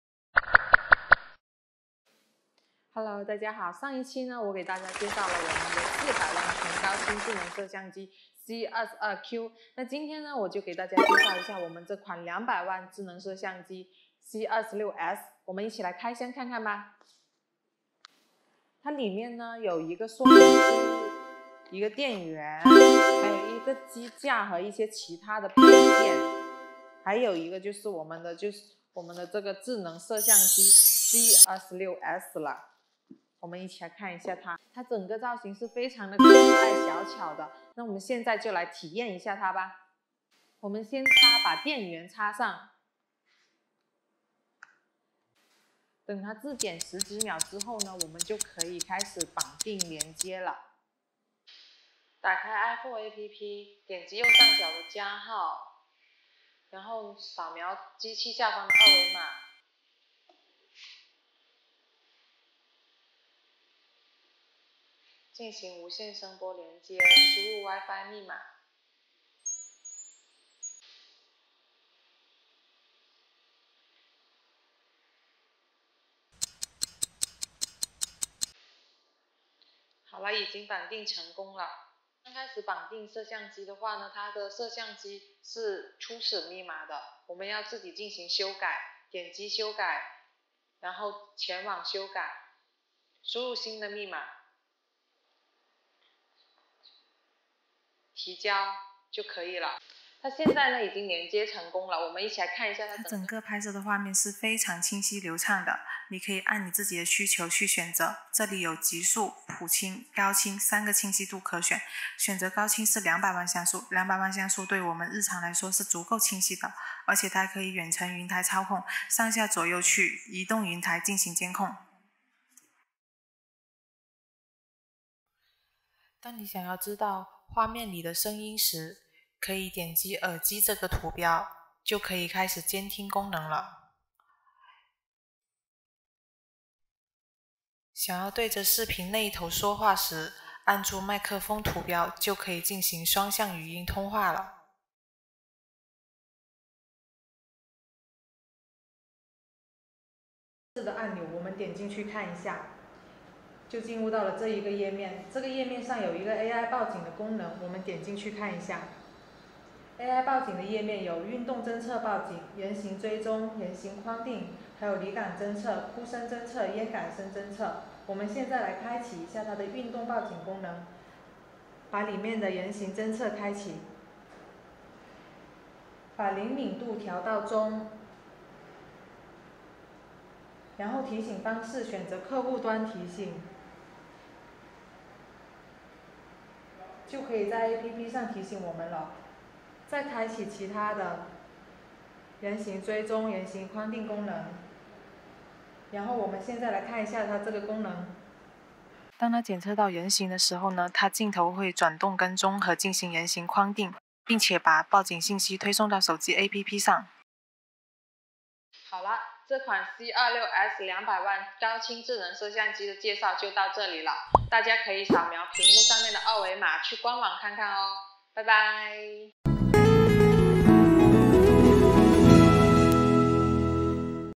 Vstarcam Intelligent high-definition PTZ camera C26S
Vstarcam Intelligent high-definition PTZ camera C26S, humanoid motion detection, smoke and whistle alarm, two-way voice, cloud storage